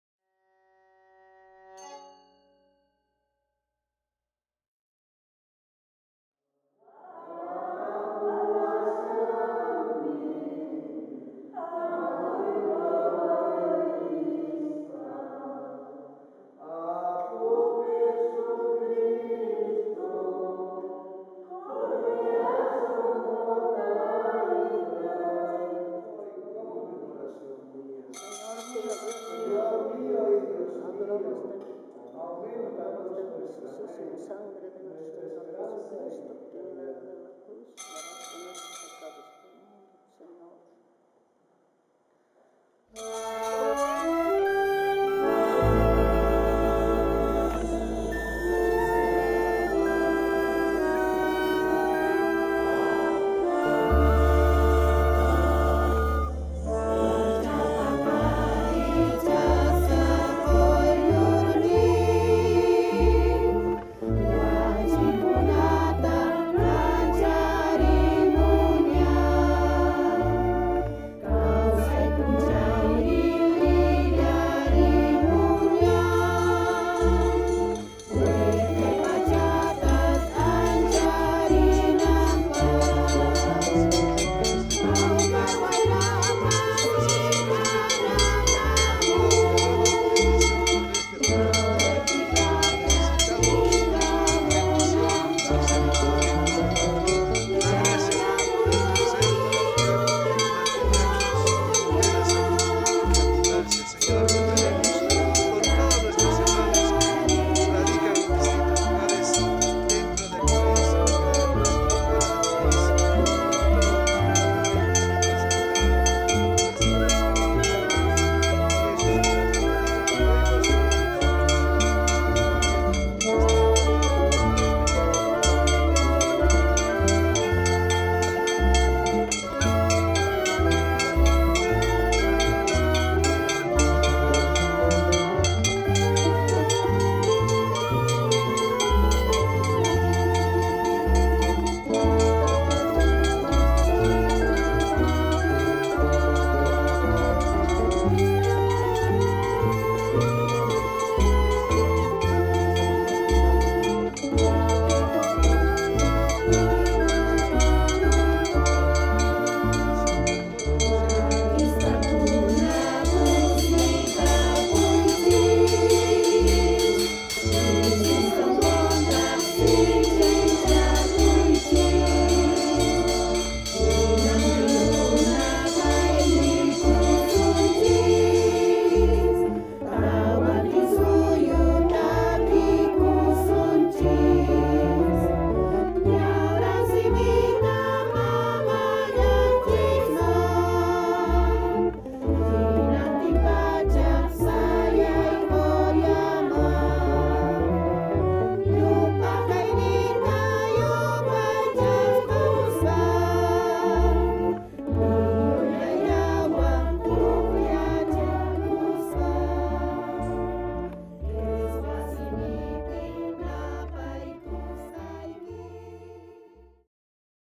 San Juan Bautista de Coya 2021
La festividad de San Juan en Coya tuvo la Bendición del Santísimo en el atrio a causa de la Pandemia del Coronavirus.